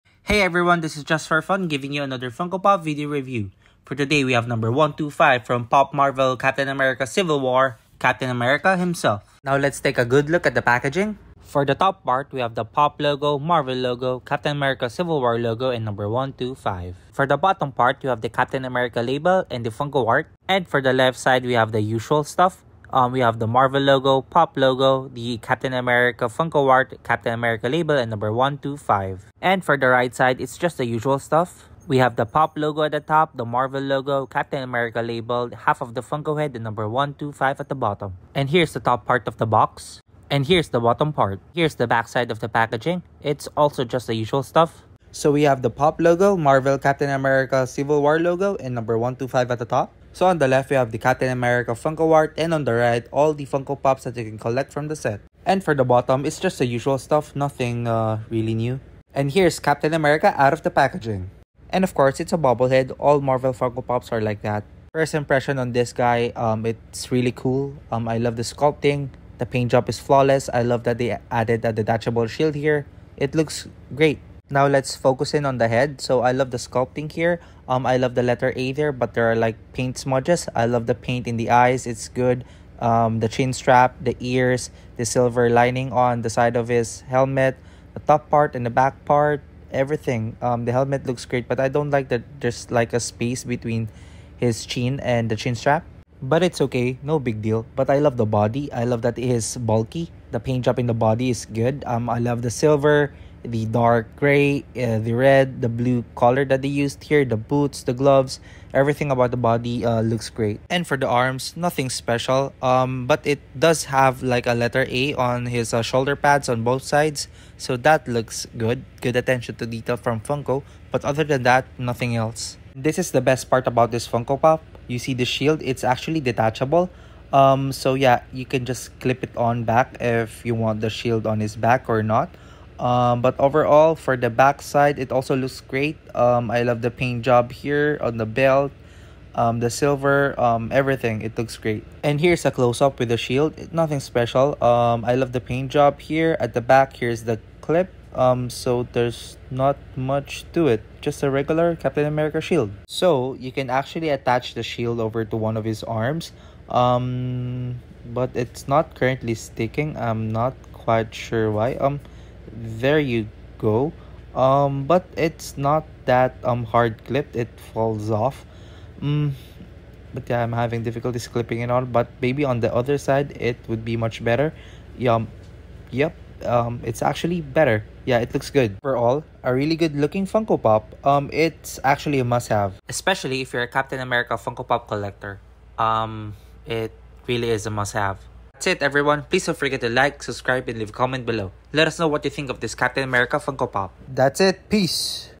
Funko Pop! Marvel: CAPTAIN AMERICA from Captain America: Civil War Unboxing and Review!
"I can do this all day." - Captain America 💪🏻

Unboxing and Review of 👉🏻 Funko Pop! Marvel: CAPTAIN AMERICA from Captain America: Civil War!